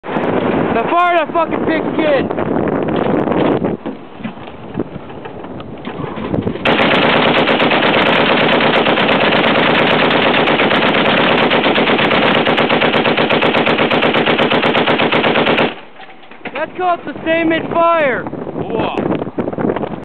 bazemore lights them up
bazemore lights up the 50 cal in germany